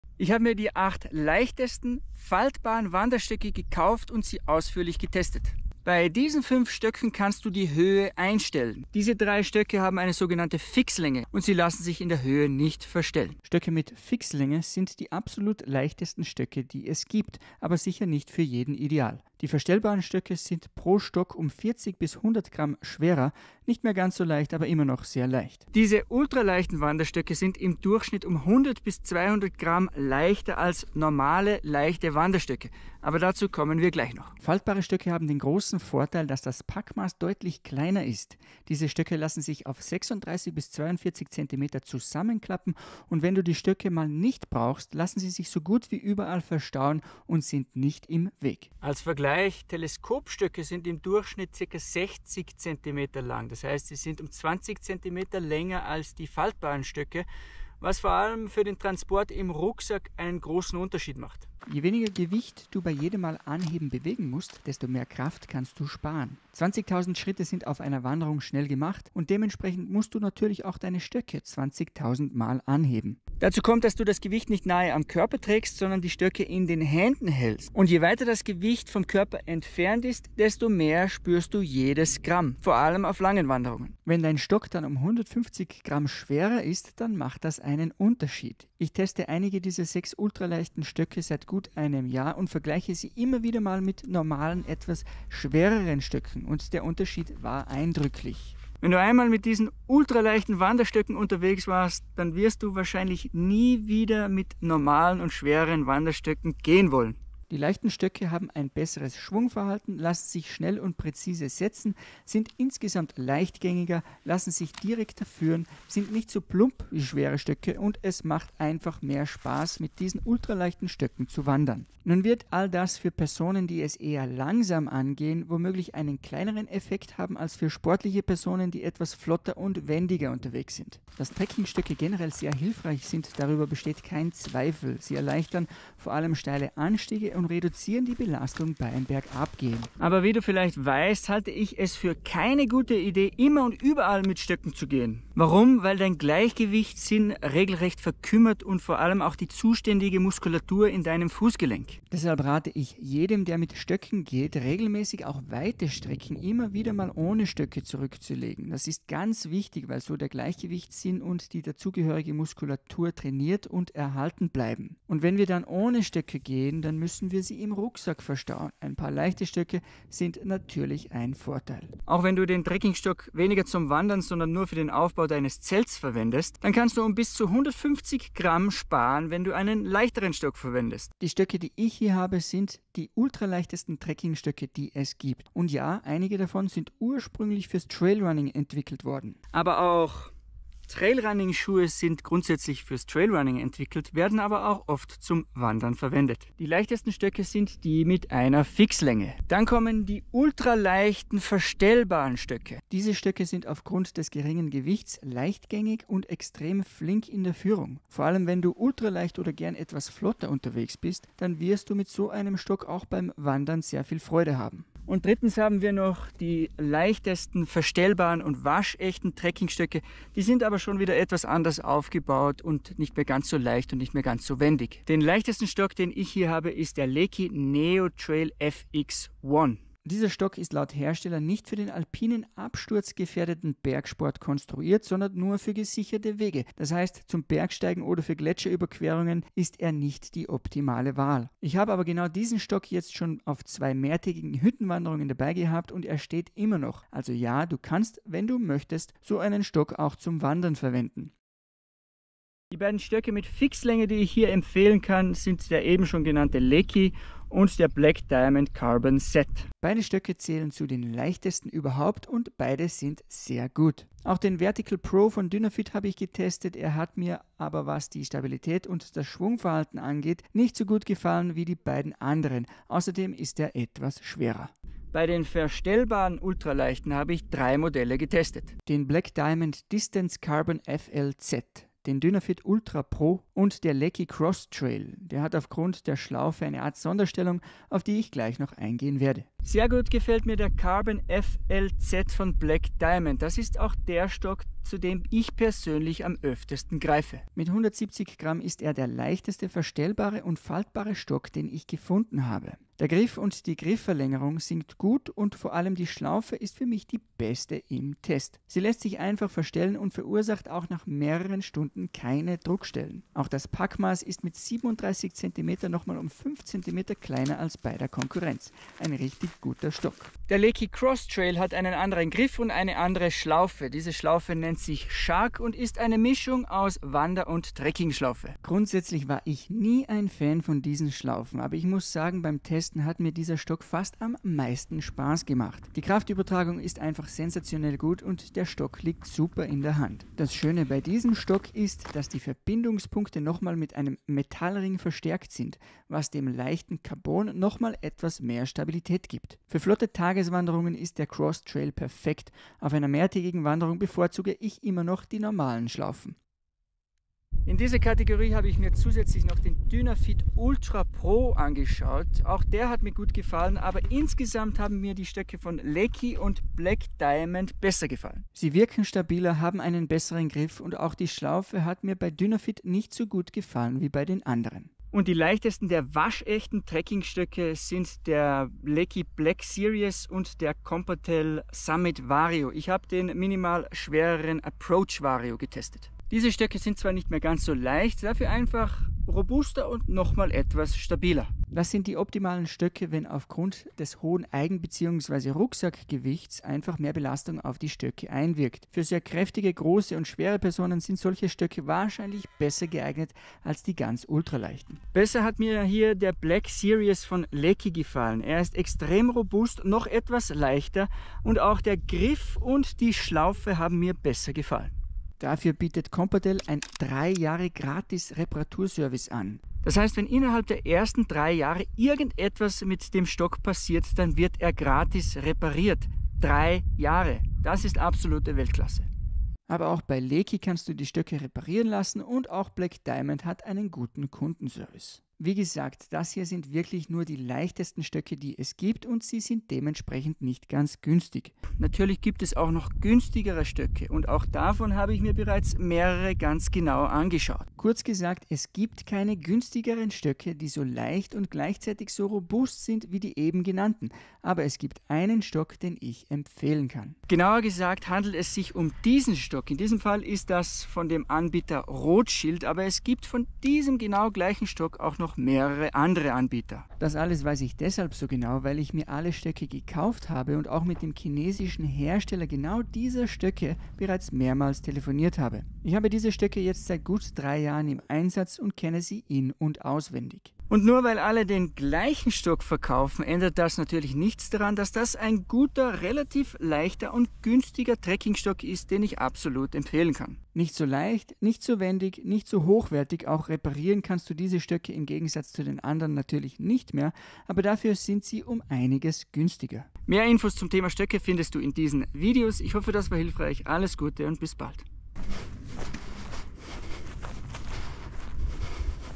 Ultraleichte Trekkingstöcke (faltbar) TEST | Das sind die BESTEN
Ultraleichte Stöcke sparen Gewicht, Kraft, haben ein kleines Packmaß und machen einfach Spaß.

Dynafit - Vertical Pro:
4. Günstige Stöcke

Sonstige Ausrüstung:
Weil ich immer am testen bin, ändert sich die Ausrüstung immer wieder mal… aber das hier ist so in etwa meine “Standart-Ausrüstung” die ich nicht immer, aber oft dabei habe.